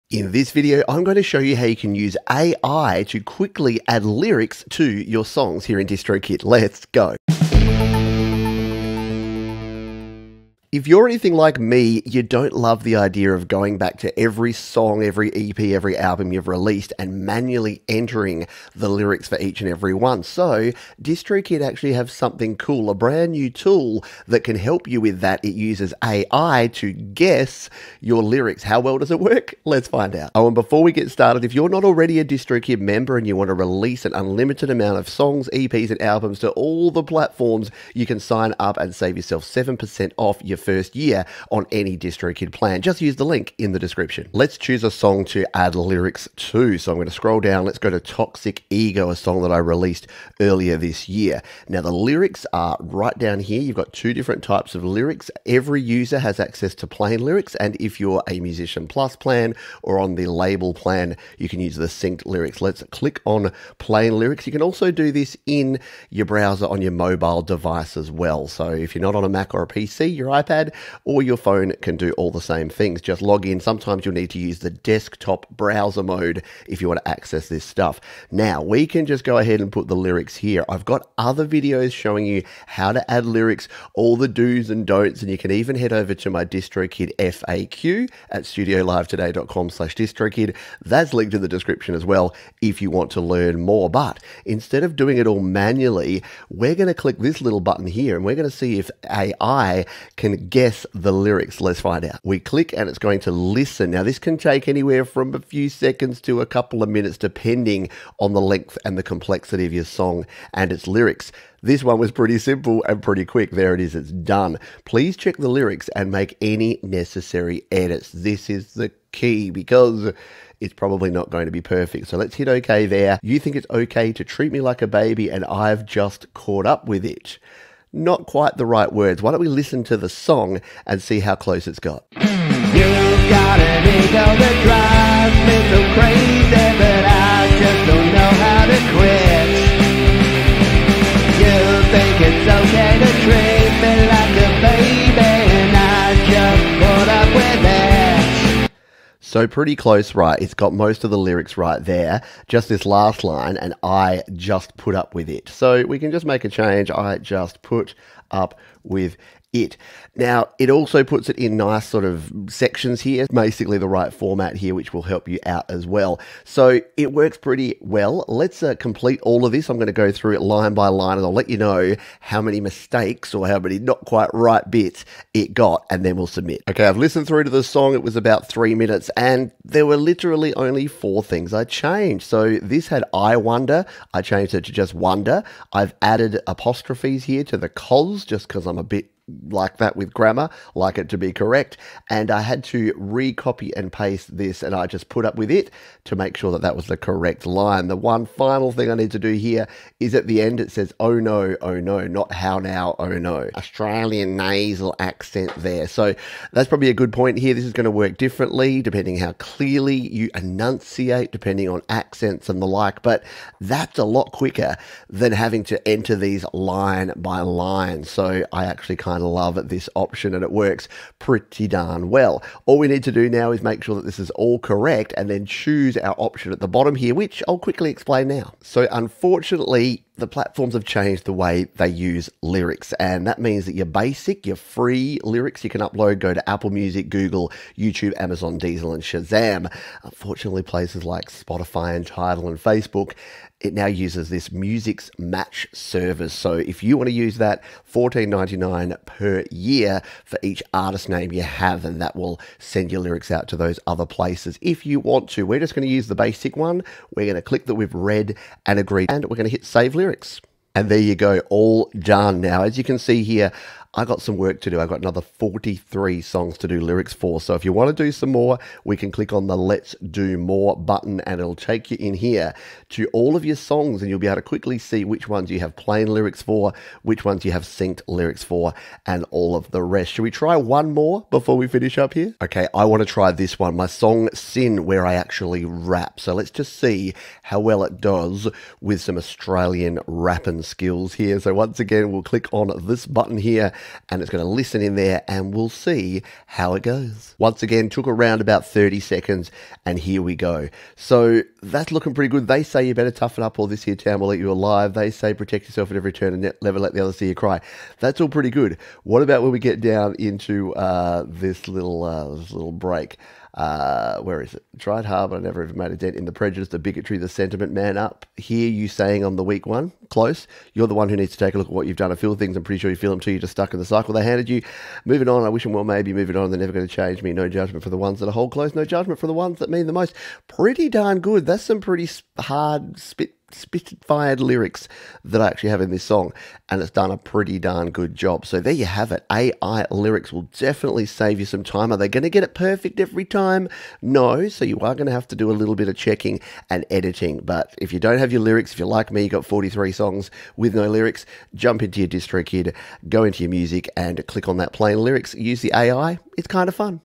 Add LYRICS in DistroKid (the easy way!)
How to use the A.I. (artificial intelligence) lyrics feature in DistroKid